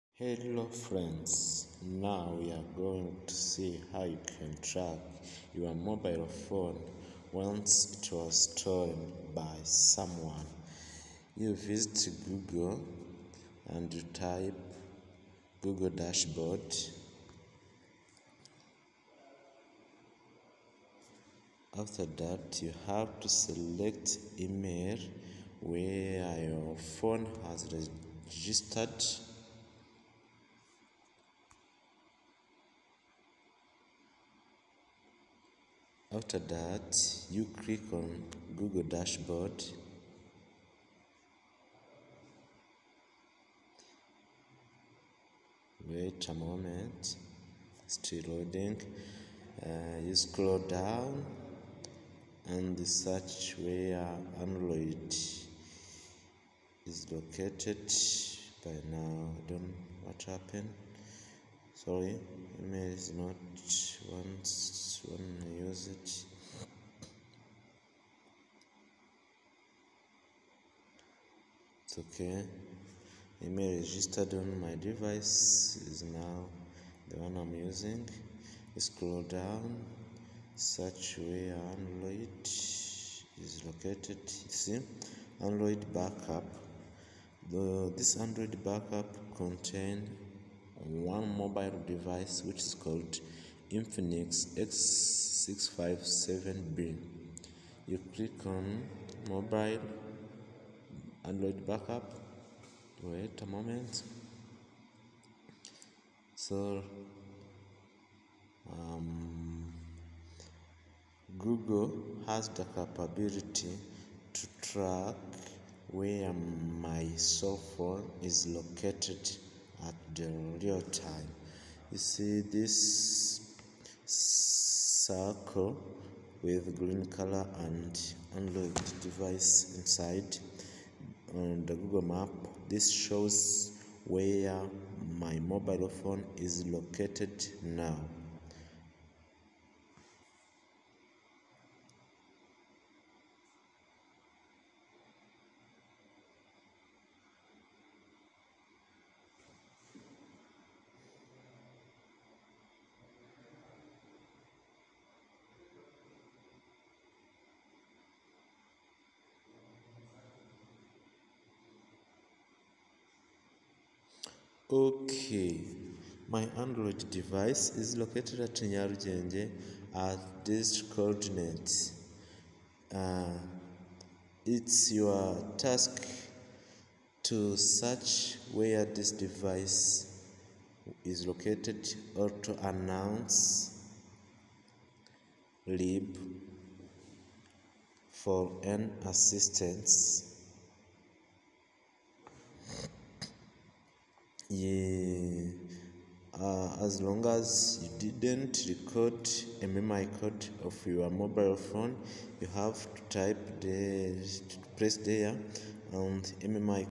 Phone tracking part1
Mobile phone tracking once it was stolen by someone (Uburyo bworoshye wabonamo phone yawe igihe bamaze kuyikwiba).